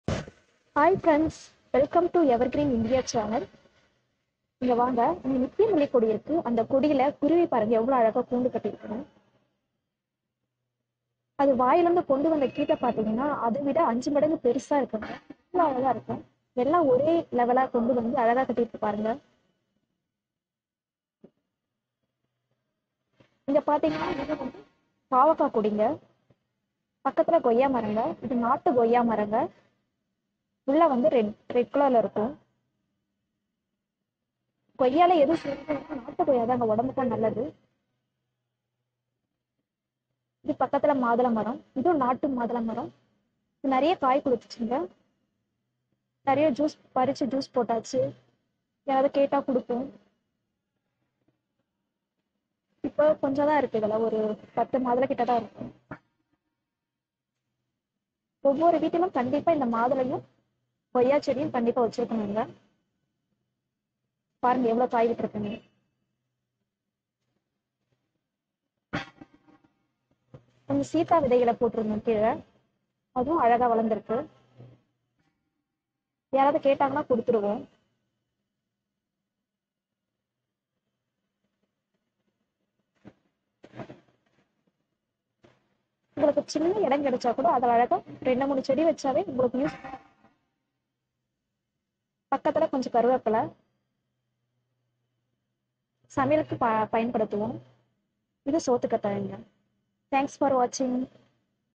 Home Garden / Veetu Thottam/ வீட்டுத் தோட்டம்
பத்து வருடங்களுக்கு மேலாக இந்த கொய்யா மற்றும் மாதுளை இரண்டு மரங்களும் கனிகளை கொடுக்கின்றன. வீட்டில் உள்ள விதைகள் மூலமாக வளர்க்கப் பட்டு நாம் ஆரோக்கியமாக வாழ மரங்கள் பெரும் பங்கு வகிக்கின்றன. நன்றி